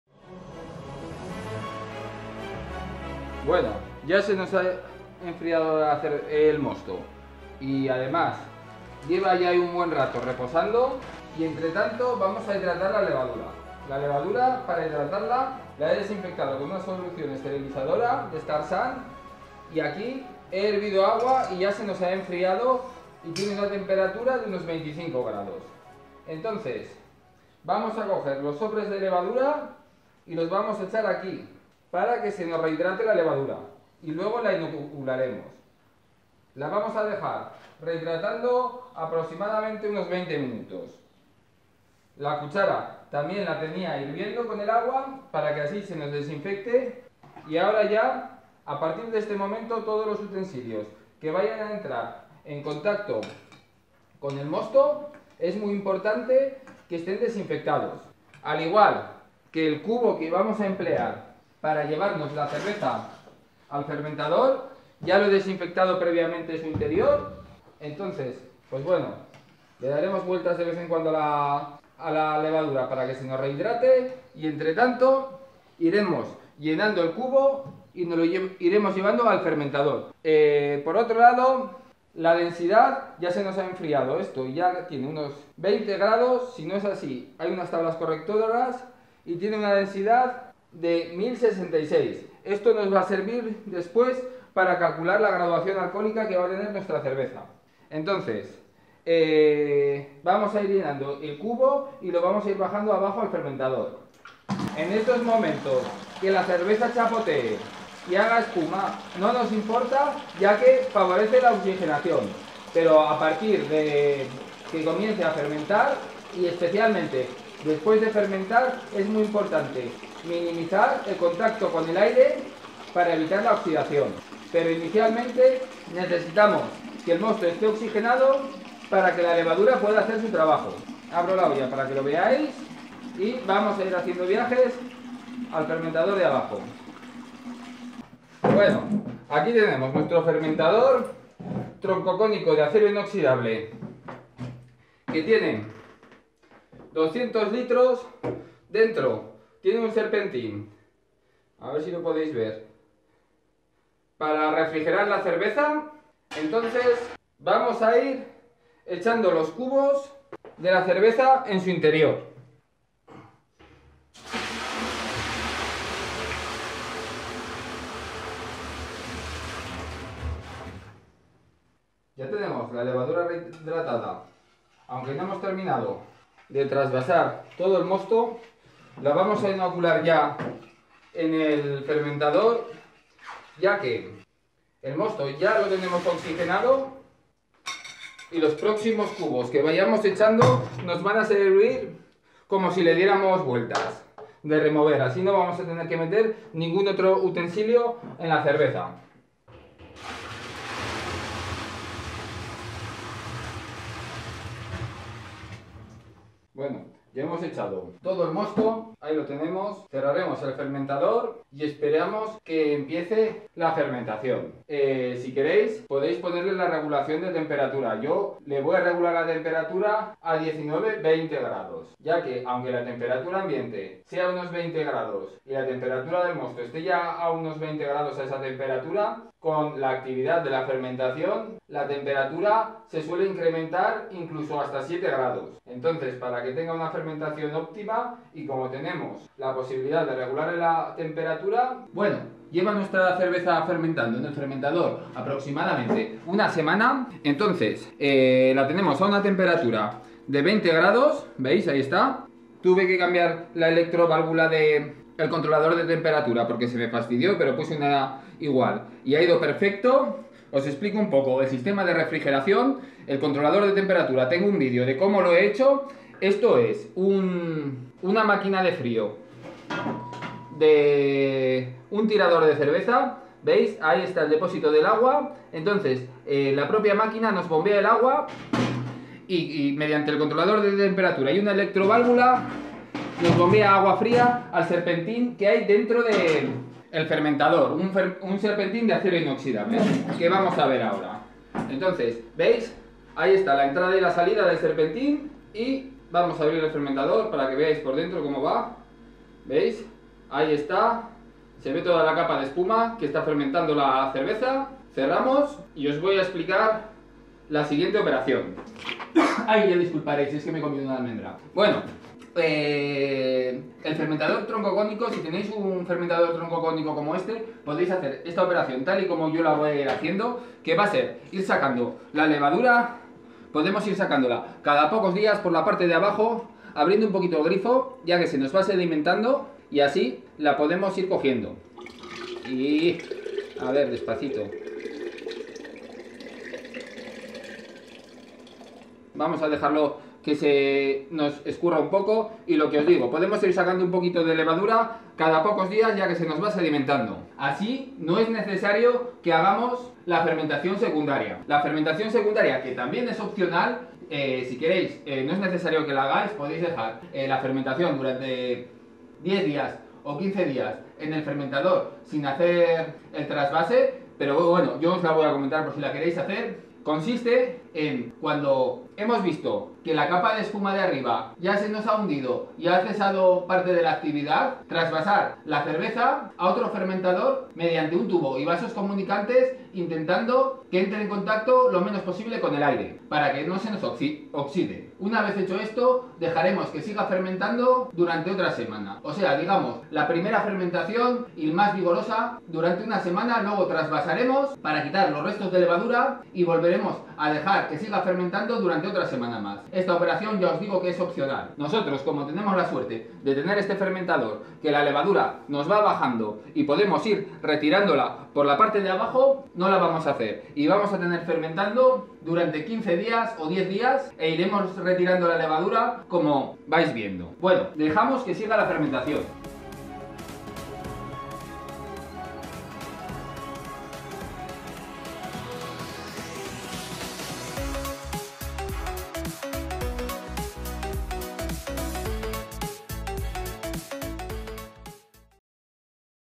Elaboracion cerveza artesana. PARTE 3
Elaboramos 100 litros de American Porter (Cerveza negra) con medios artesanales explicando cada procedimiento así como sus principios para que resulte util tanto a principiantes como a inciados.

Los ingredientes, cantidades y otros datos precisos los podeis encontrar en la
Receta:

Tambien os dejo unas
Hojas de control de procesos para anotaciones:

Los datos de la elaboración:
D.I.(Densidad relativa antes de fermentar) 1066.
D.F.(Densidad relativa Despues de fermentar) 1010.
7,11 % Vol. (Graduación alcoholica)
Volumen de la elaboración 100 Litros.

Enlaces de compra de materiales y utensilios:
Materias primas y utensilios:

Fermentador de plastico 30 l con airlock y valvula.

Densimetro y probeta:

Calculadora de cerveza:
Calculadora de grado Alcoholico:

Tutoriales Cosas de Campo